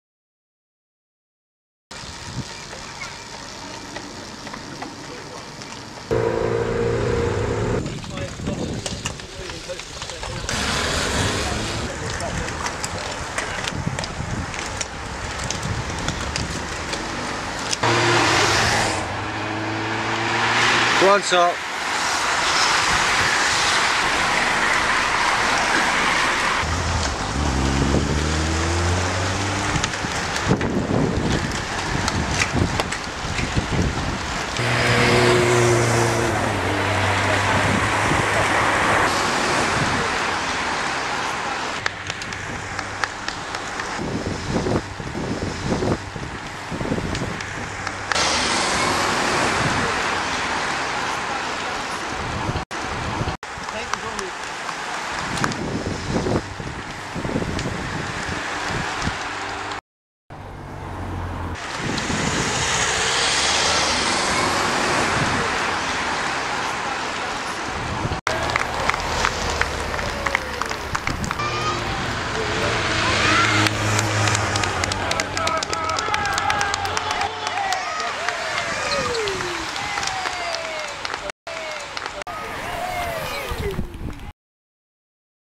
South Bristol Road Race - April 2013
Cat 3/4 58 mile Road Race at Bishop Sutton on Sunday 21st April 2013